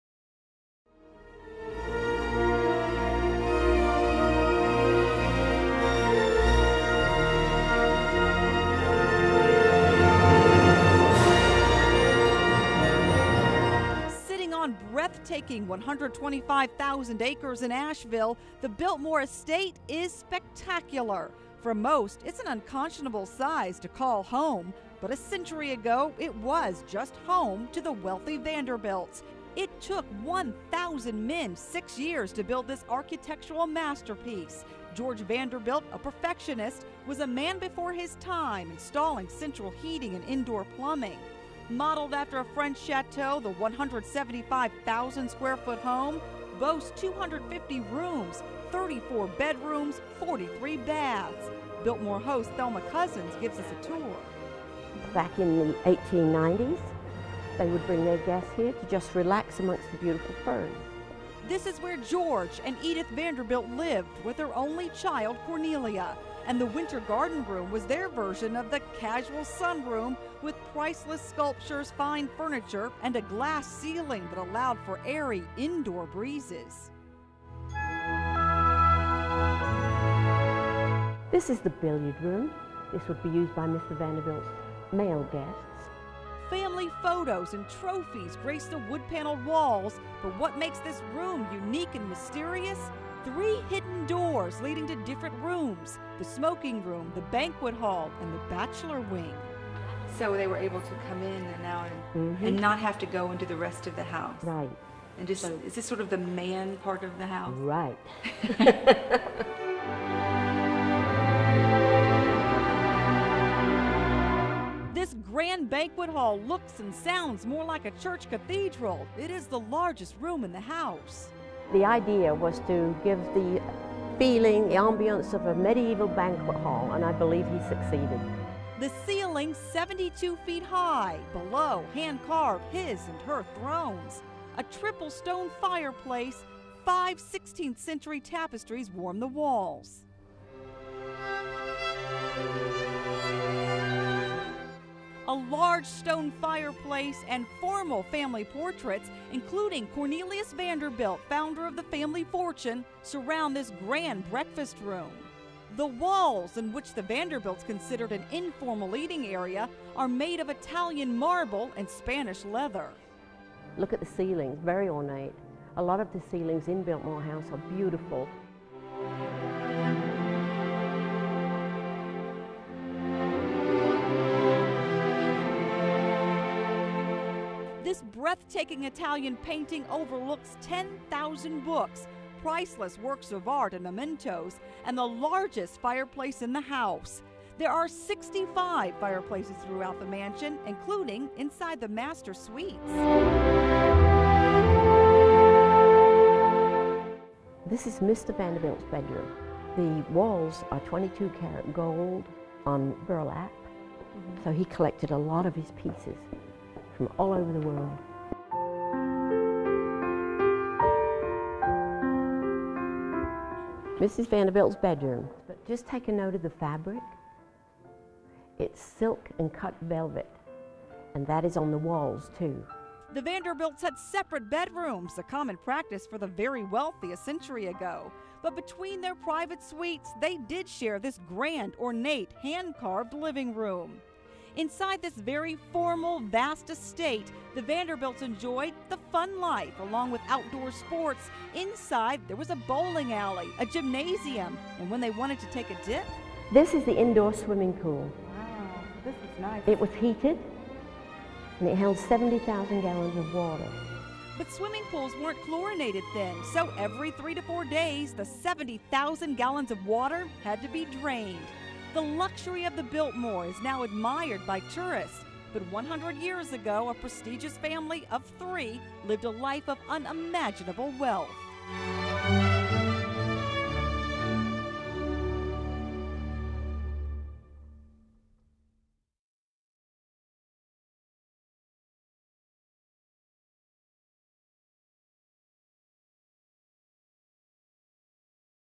Biltmore Estate Profile
This piece is one out of a series of pieces profiling luxurious homes in North Carolina.  While most of the pieces were with an "MTV Cribs" influence, profiling a regal estate such as the Vanderbilt Mansion in Asheville, NC required a more stately approach.  I hope you like it.